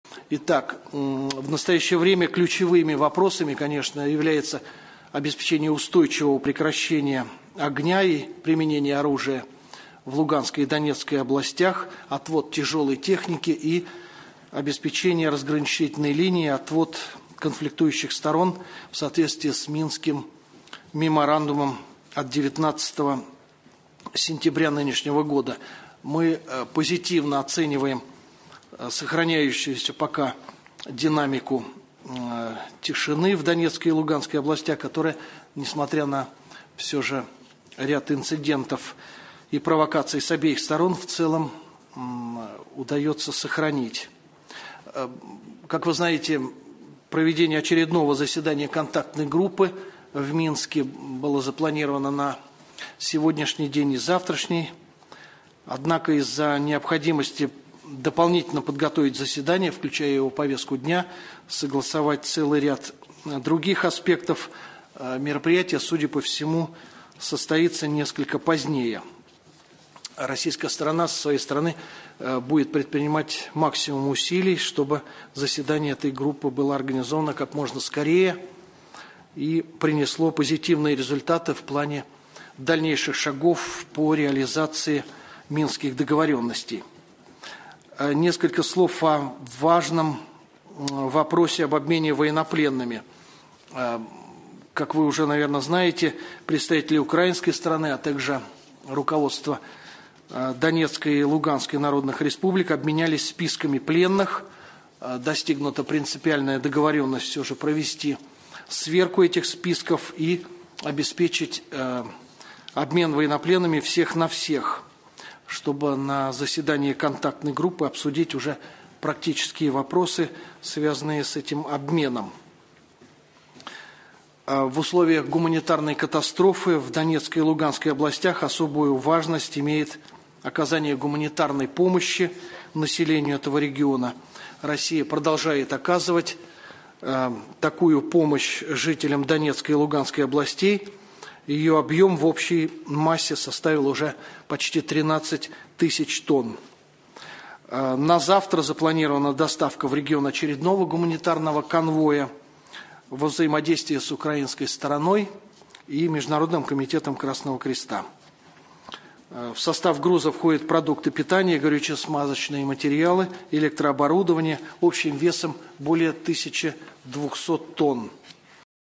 опорные тезисы России по Украине 11.12.2014 МИД России
1. В настоящее время ключевыми вопросами являются прекращение применения оружия в Донецкой и Луганской областях, отвод тяжелой техники, а также определение линии разведения конфликтующих сторон в соответствии с Минским меморандумом от 19 сентября с.г.

Позитивно оцениваем объявление и динамику соблюдения режима «тишины» в Донецкой и Луганской областях, который, несмотря на провокации с обеих сторон, в целом сохраняется.

2. Как вам известно, проведение очередного заседания Контактной группы было запланировано на 11-12 декабря в Минске. Однако из-за необходимости дополнительно проработать его отдельные аспекты, включая повестку дня, мероприятие, судя по всему, состоится несколько позднее.

Российская сторона предпринимает максимум усилий для организации заседания Контактной группы в кратчайшие сроки и достижения позитивных результатов для реализации минских договоренностей.

3. Важное значение имеет решение вопроса об обмене военнопленными. Представители украинской стороны, а также руководство ДНР и ЛНР обменялись списками пленных. Между ними достигнута договоренность провести сверку переданных списков, чтобы на заседании Контактной группы обсудить практические вопросы, связанные с обменом военнопленными.

4. В условиях гуманитарной катастрофы в Донецкой и Луганской областях особую важность имеет оказание гуманитарной помощи населению этого региона. Объем российской помощи жителям Донецкой и Луганской областей составил 12985 тонн. На 12 декабря запланирована доставка в регион очередного гуманитарного конвоя во взаимодействии с украинской стороной и Международным Комитетом Красного Креста. В состав груза входят продукты питания, горюче-смазочные материалы, электрооборудование общим весом более 1200 тонн.

Брифинг официального представителя МИД России А.К.Лукашевича, 11 декабря 2014 года